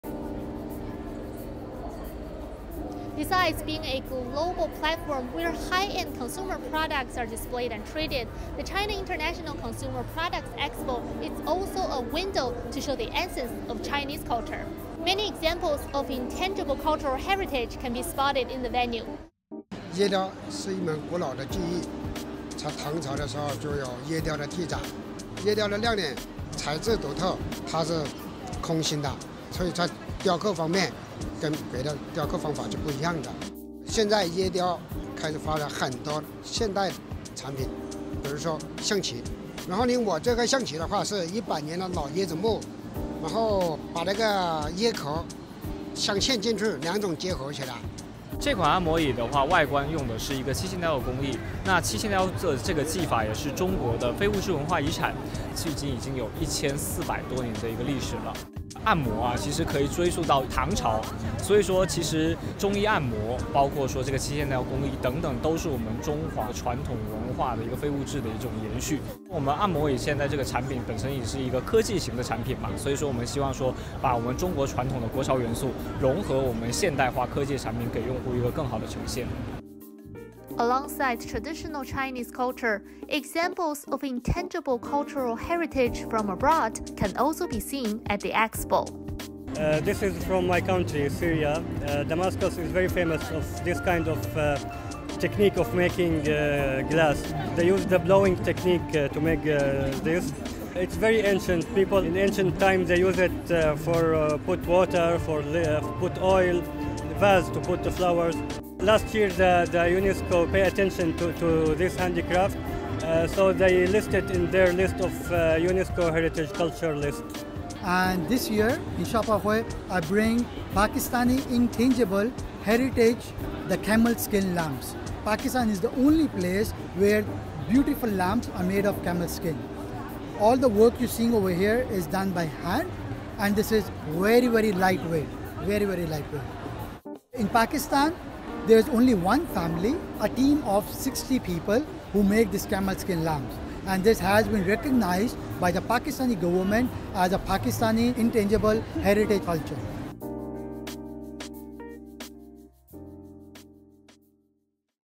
Vlog: High-end consumer products expo meets essence of culture
Besides being a global platform where high-end consumer products are displayed and traded, the China International Consumer Products Expo is also a window to show the essence of culture. Many examples of intangible cultural heritage can be spotted in the venue.

Join CGTN's Guo Meiping, check out the exquisite craftworks of intangible cultural heritage, and learn about culture behind them.